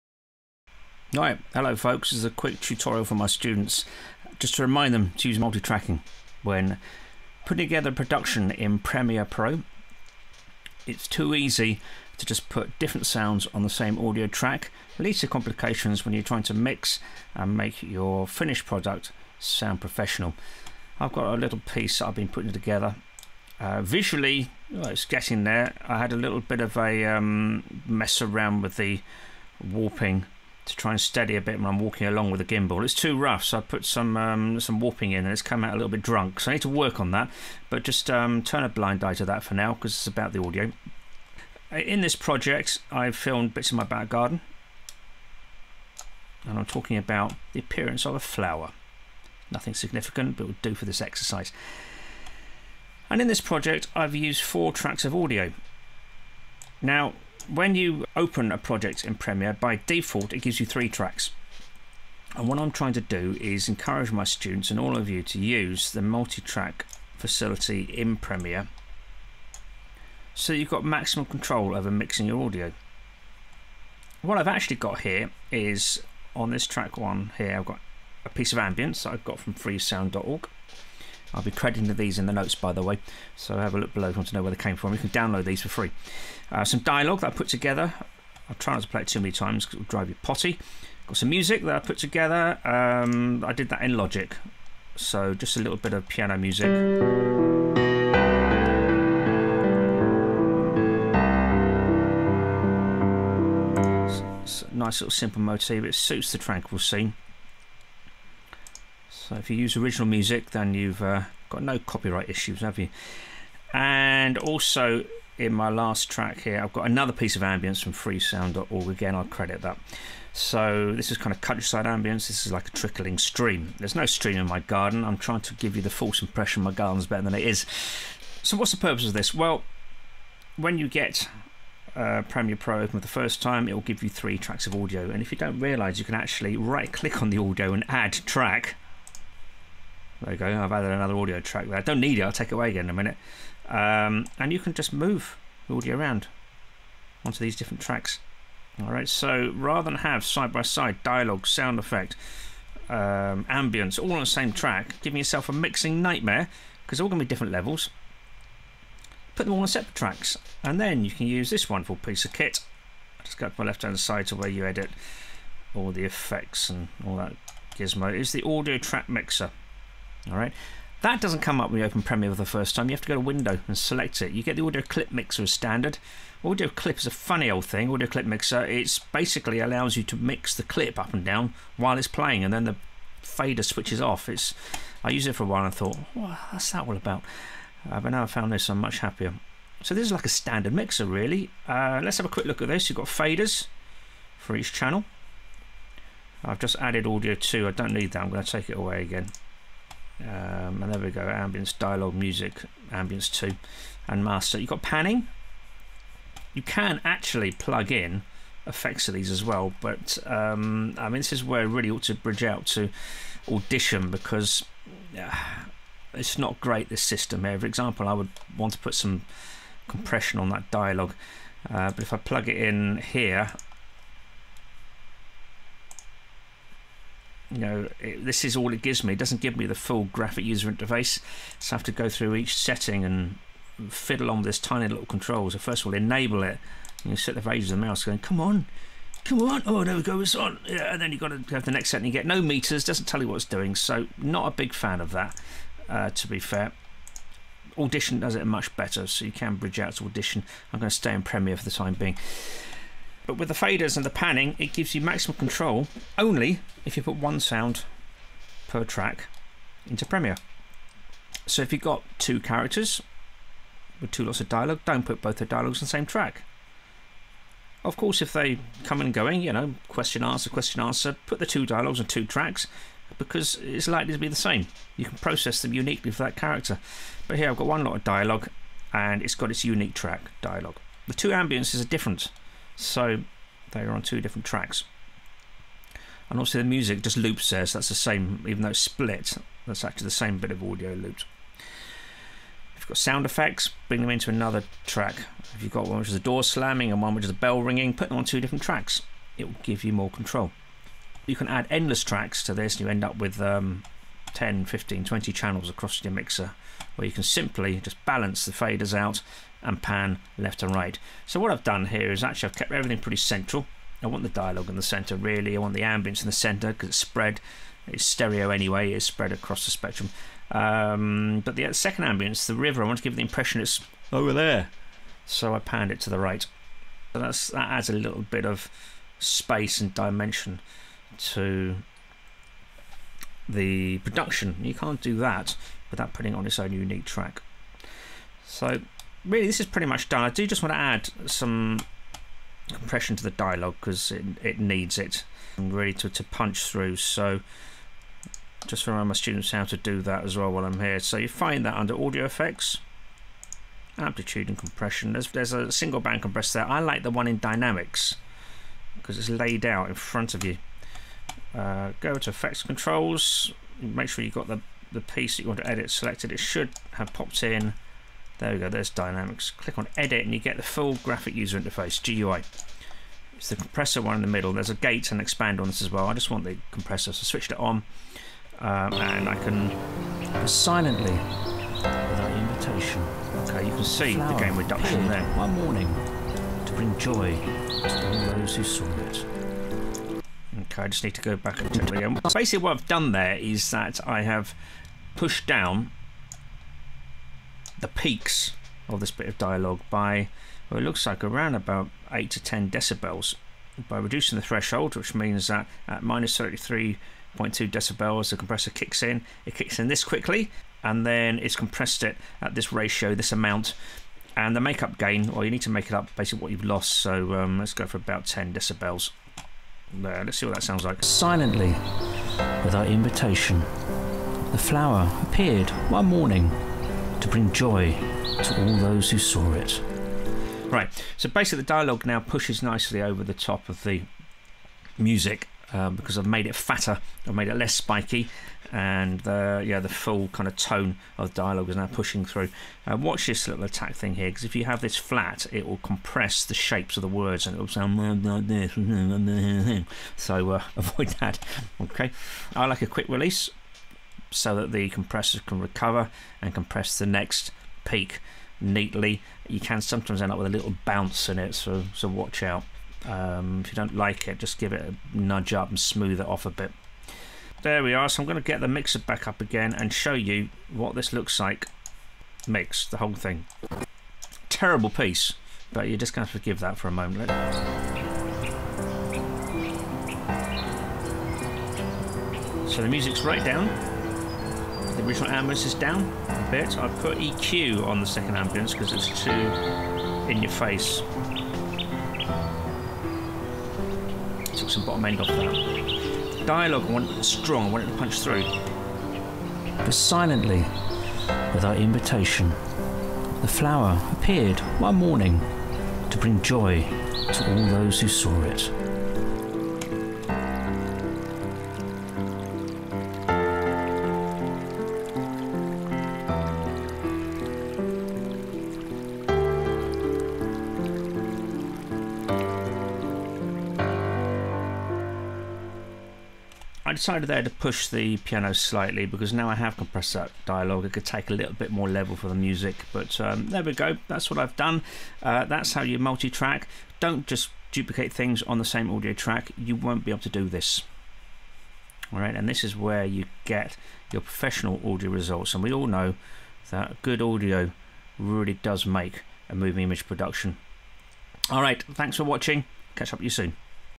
Multitrack Audio Mixing In Adobe Premiere Pro CC
Tutorial for my student group on mixing audio for their moving image projects. Shows how to allocate each unique audio element to its own track; how to compress dialogue, and how to use the Audio Track Mixer to balance and pan audio tracks.

All original content except for two ambient tracks available on freesound.org

Thank you Rambler 52 for the country sounds
"Stream, Water, C.wav" by InspectorJ of Freesound.org